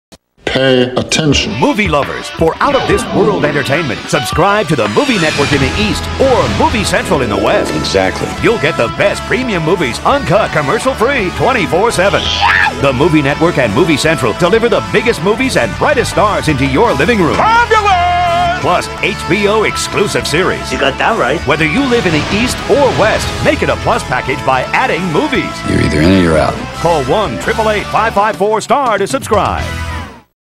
Movie Central/The Movie Network- National Spot
This is a small example of a national spot I wrote/produced and directed.